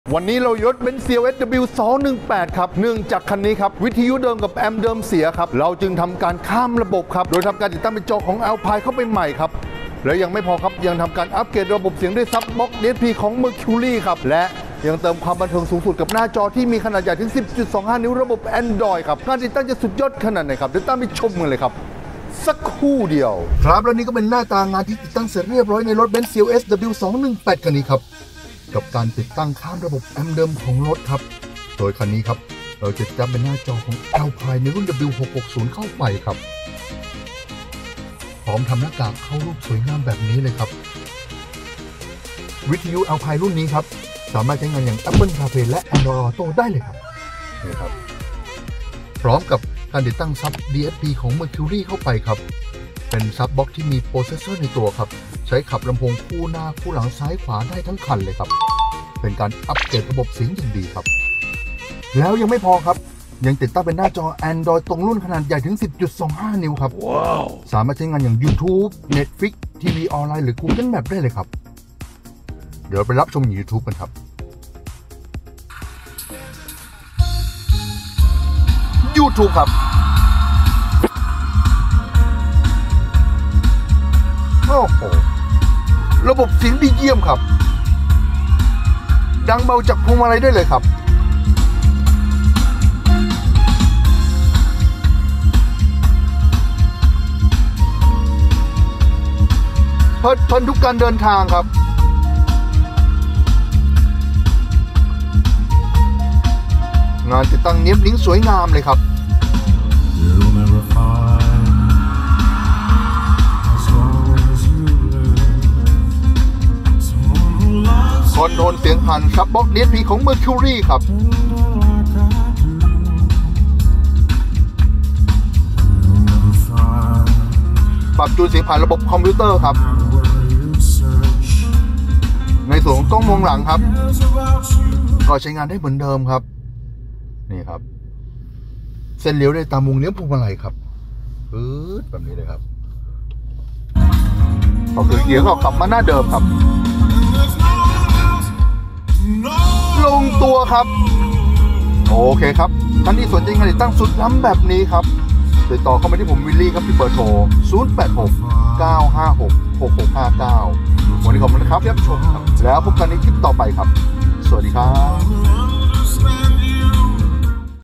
อัพเกรดข้ามระบบวิทยุเดิมเป็น จอ ALPINE iLX-W660E บนรถ BENZ CLS W218 ขนาด 10.25 นิ้ว by Willy Mirage
ถ้า Amp เดิมของรถเสีย ❗️ต้องทำยังไง ❓
🟣 BENZ CLS63 W218
** คันนี้เนื่องจากแอมป์ตัวเดิมเสีย เราจึงทำการข้ามระบบแอมป์เดิม
โดยใช้เป็นแอมป์ใหม่ จาก SUBBOX DSP MERCURY
และติดตั้งเป็นจอแอนดรอยด์ตรงรุ่น🔺
✔️ วิทยุ ALPINE พร้อมติดตั้งหน้ากากใหม่
✔️ พร้อมจอแอนดรอยด์ตรงรุ่น BENZ สามารถใช้งานร่วมกับวิทยุได้
รองรับการใช้งานแอพพลิเคชั่นต่างๆ สามารถดาวน์โหลดแอพพลิเคชั่นต่างๆ
ไปบนหน้าจอ และเพลิดเพลินกับ content Online มากมาย
: YouTube / NetFlix / TvOnline / Spotify
✔️ MERCURY DSP 8.4 HD : BASSBOX ที่มี PROCESSOR ในตัว
เทคโนโลยีแบบ All in One ที่ครบจบในตัวเดียว!
◼️ SUBBOXอัจฉริยะ มาพร้อมระบบขยายเสียง และ Digital Sound Processor ในตัว
◼️ ปรับแต่งเสียงได้หลากหลายมิติ มอบเสียงเบสที่หนักแน่น กระชับ
เก็บรายละเอียดครบถ้วน ตอบสนองทุกย่านความถี่ให้คุณสัมผัสมิติใหม่แห่งเสียงเพลง
◼️ ปรับจูนเสียงด้วยระบบ EQ สามารถปรับแต่งเสียงได้ พลังขับเสียงดีเยี่ยม
พร้อมทำแผงครอบปิดเรียบร้อย และสวยงาม
📌MIRAGEAUDIO สาขา1📌
🕚 เปิดบริการ วันอังคาร-วันอาทิตย์
เวลาทำการ 9:00 - 19:00 (หยุดทุกวันจันทร์)
📌 MIRAGE AUDIO1 แผนที่นำทาง
⚫️ Tiktok : @Willy Mirage Audio (@willymirage)